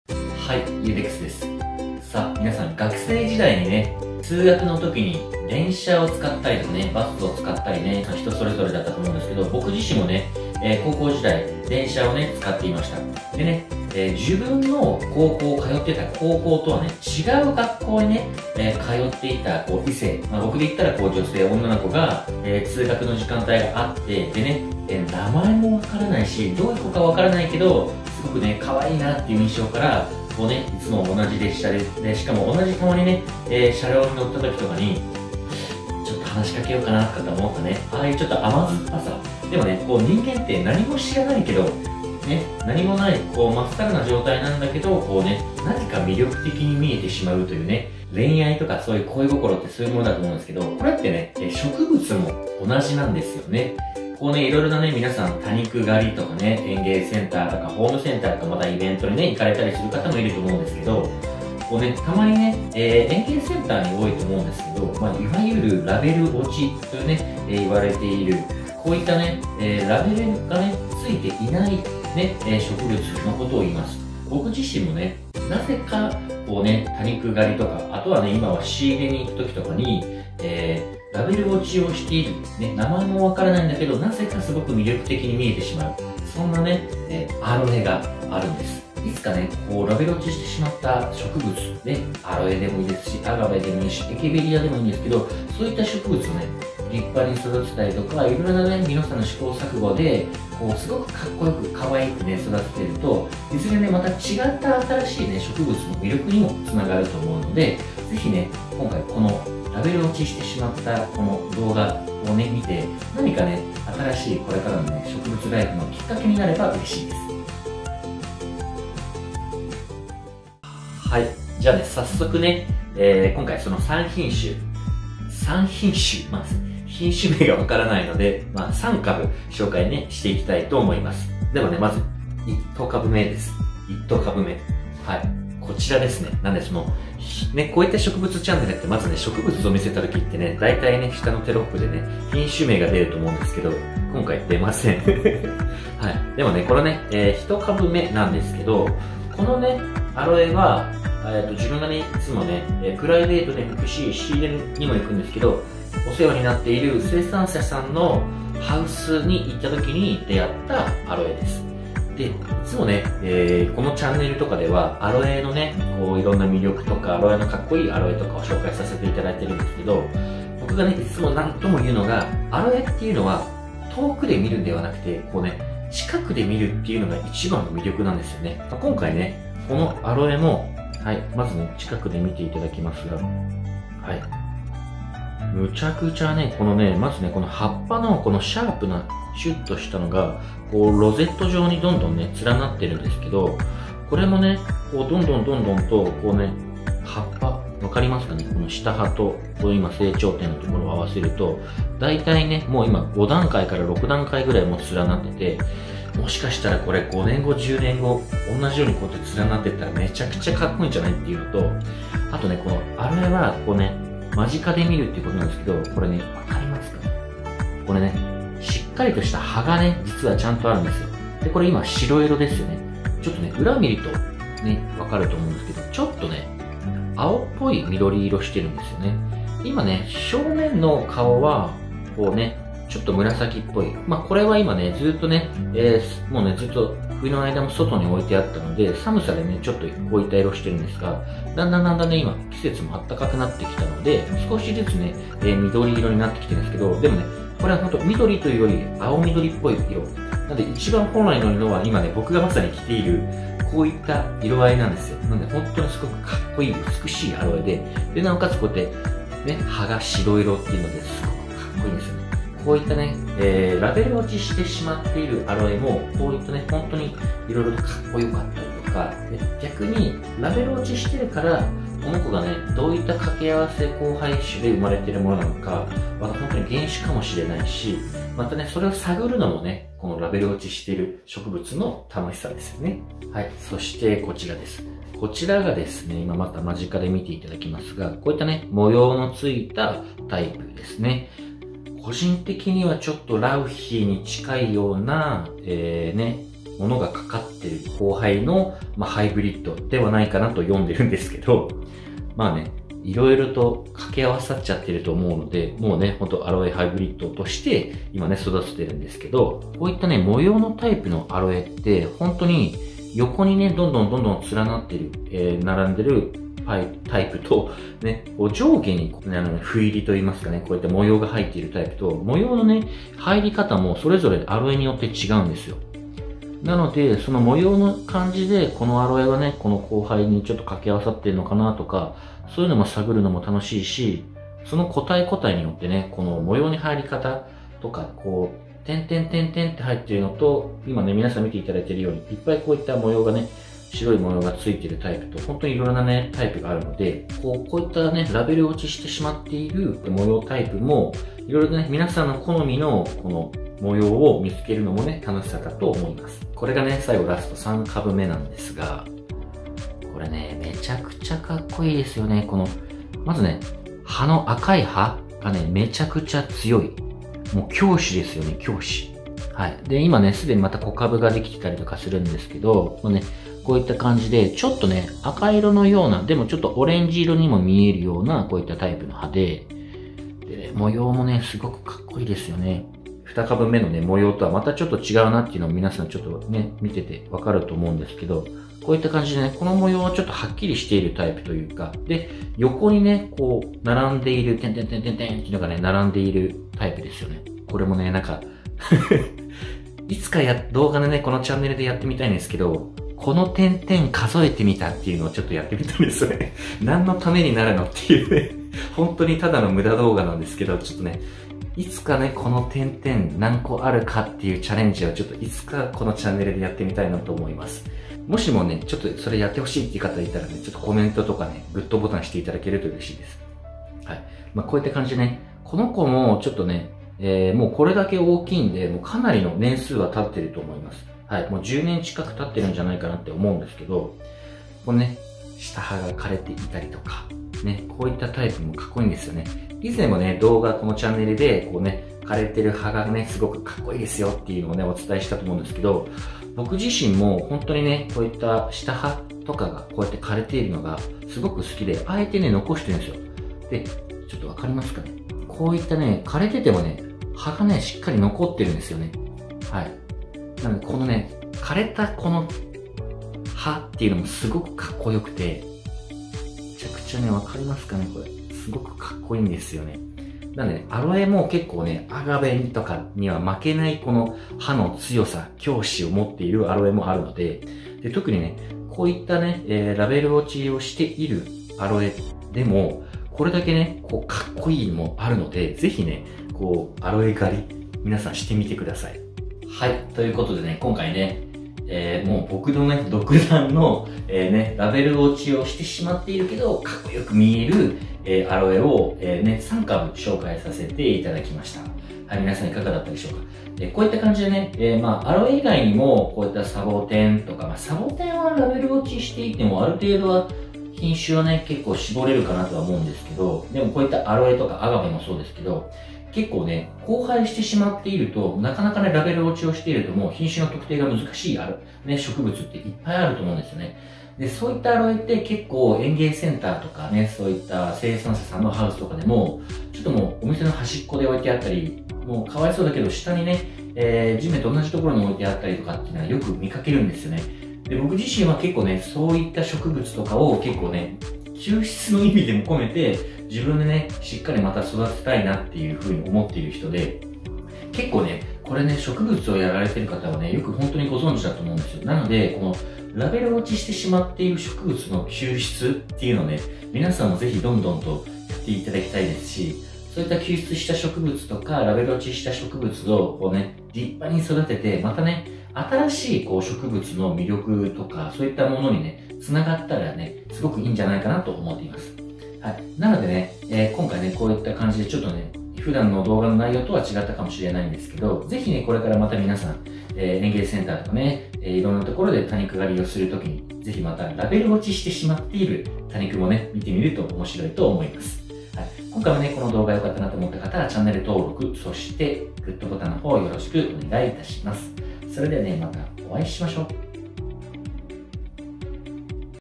【奇跡】衝撃のアロエを目の当たりにするのはあなたです
YUDEXです。

名前もわからない株
そんな株、皆さんもおうちにありますよね
新しい多肉ライフのきっかけなるかも